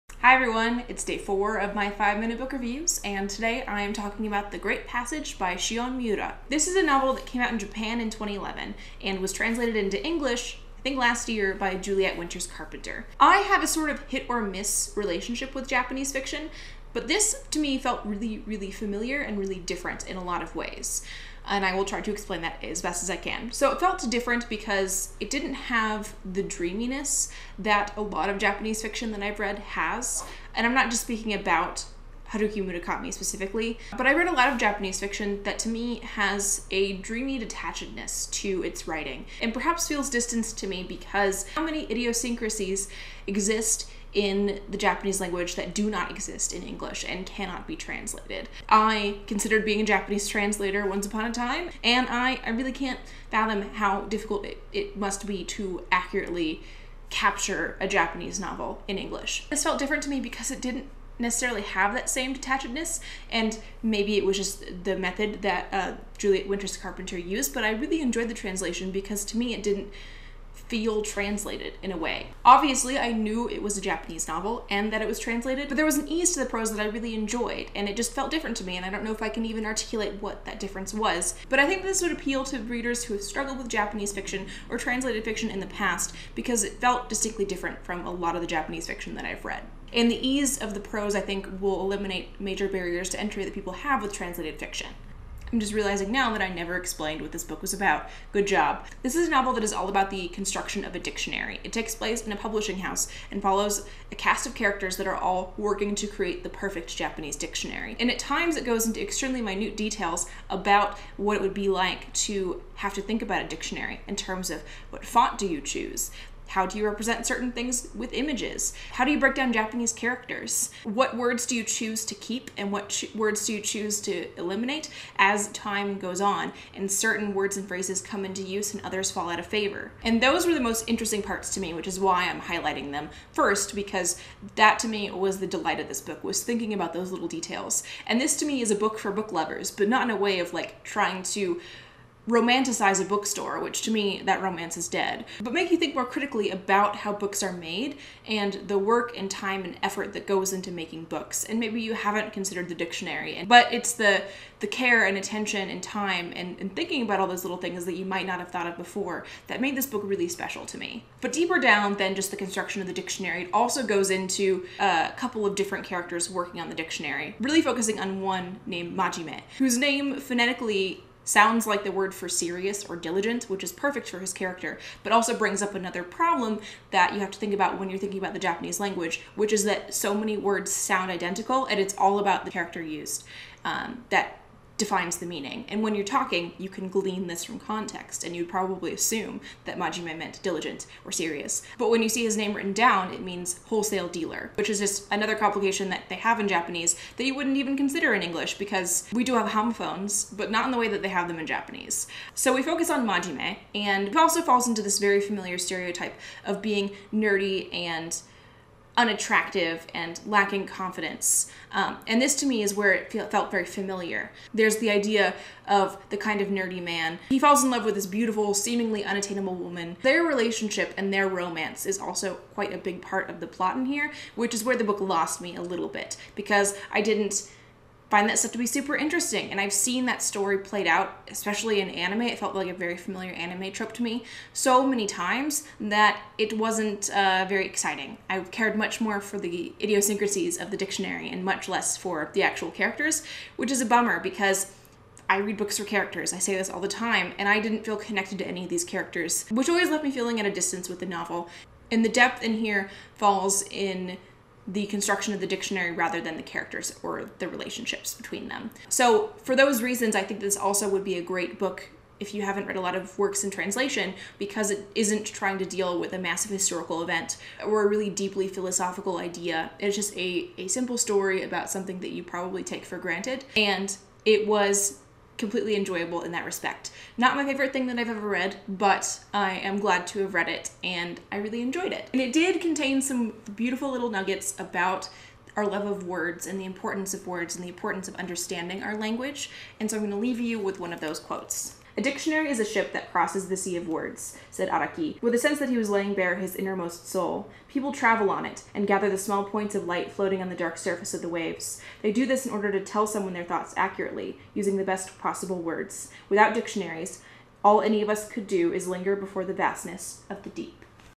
Book Review | The Great Passage
My thoughts on the Japanese novel The Great Passage.

I also thought while editing that perhaps this translation felt different because of the translator's choice to leave common Japanese words untranslated, which was a unique and welcome facet to the reading experience. Something fun to think about.